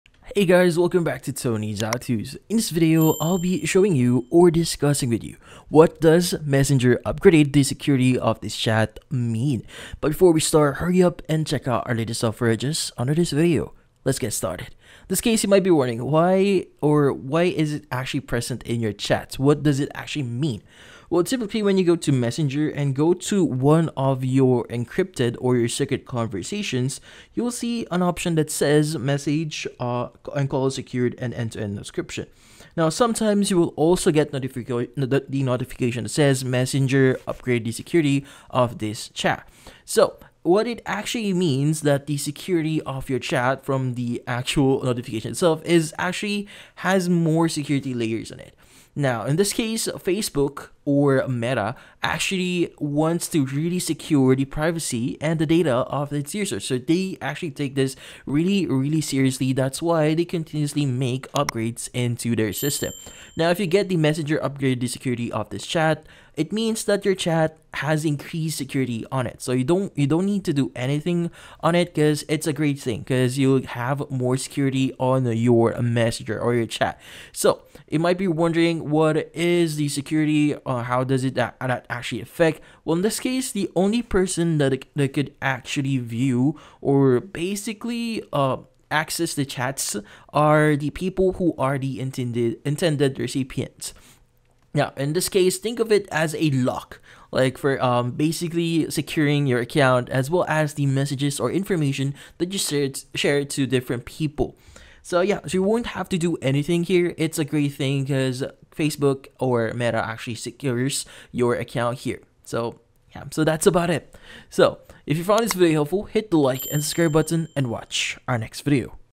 What Does ‘Messenger Upgraded the Security of This Chat’ Mean (2024)
In this video, I show you What Does ‘Messenger Upgraded the Security of This Chat’ Mean in 2024. The quick and easy tutorial Facebook messenger upgraded the security of this chat meaning. Do you want to know step by step how to fix messenger upgraded the security of this chat problem in 2024? Yes? Great!